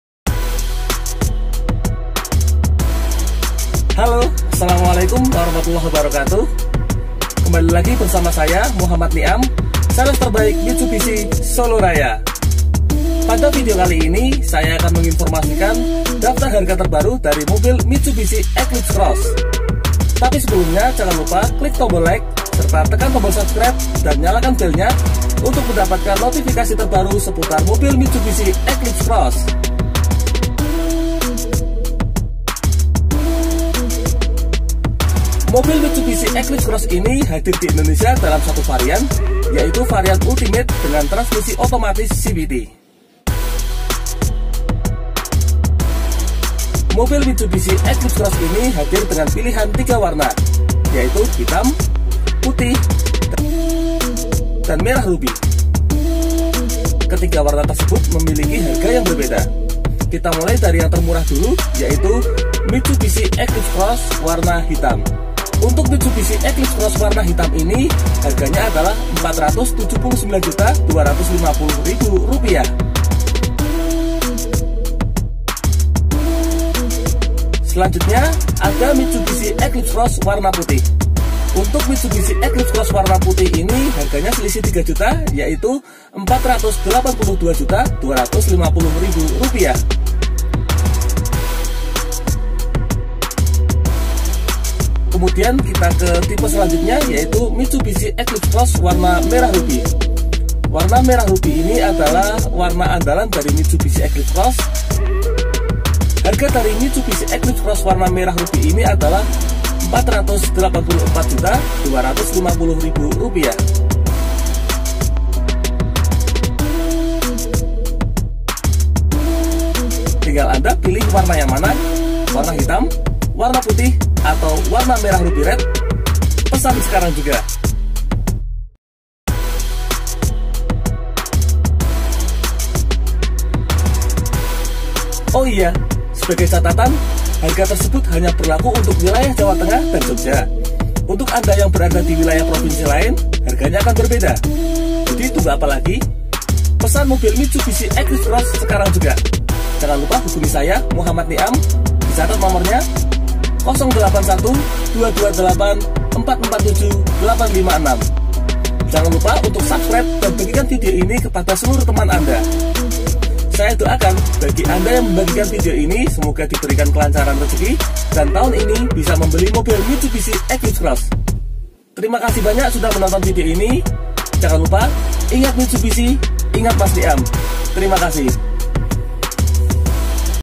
Daftar Harga Mitsubishi Eclipse Cross Terbaru - Juli 2019 Wilayah Jawa Tengah Solo
Pada video kali ini saya menginformasikan Daftar Harga terbaru mobil Mitsubishi Eclipse Cross. Untuk mobil Mitsubishi Eclipse Cross ini hanya tersedia dalam satu varian yaitu Ultimate dengan transmisi otomatis CVT.

Mobil Mitsubishi  Eclipse Cross ini tersedia dalam 3 warna yaitu Hitam, Putih dan Merah Ruby, ketiganya memiliki harga yang berbeda:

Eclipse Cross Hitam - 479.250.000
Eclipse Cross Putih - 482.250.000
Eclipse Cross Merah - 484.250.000

Harga tersebut berlaku untuk Wilayah Jawa Tengah dan Jogjakarta. Untuk informasi lebih lanjut, hubungi:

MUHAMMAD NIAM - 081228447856